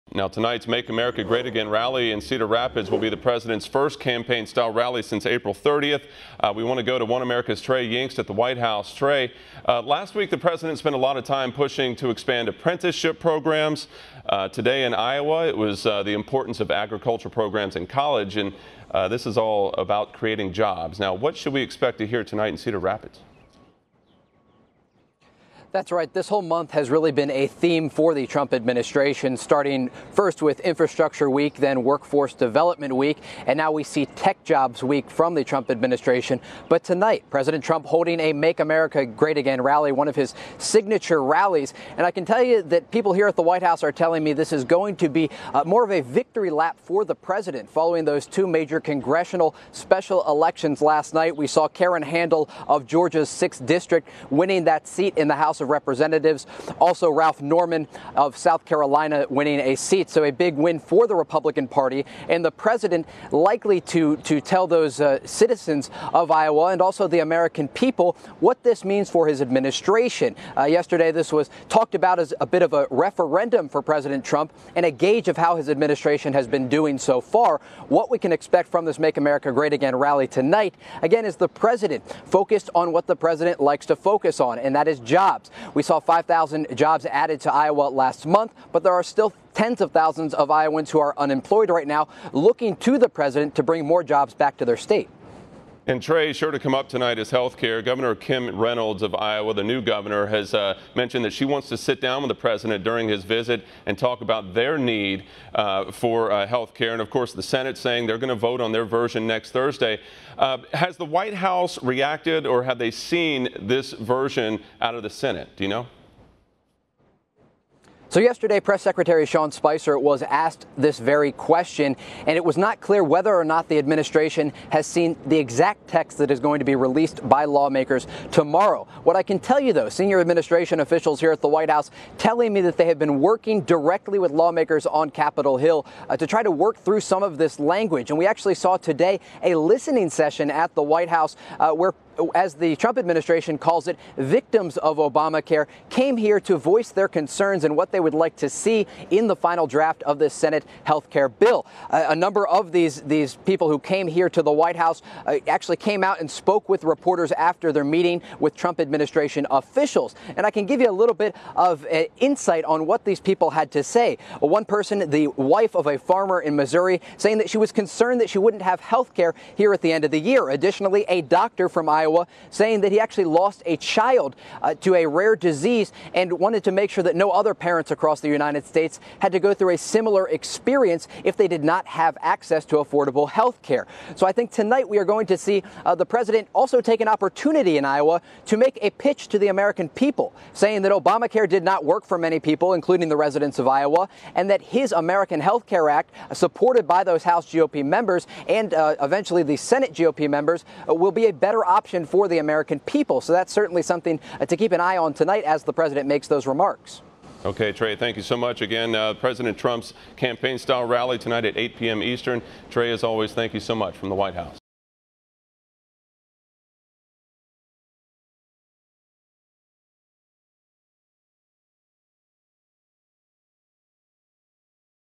President Trump Holds Rally in Iowa
President Trump traveled to Iowa Wednesday to promote technology week and talk about job creation. One America's Chief White House Correspondent Trey Yingst has more from the White House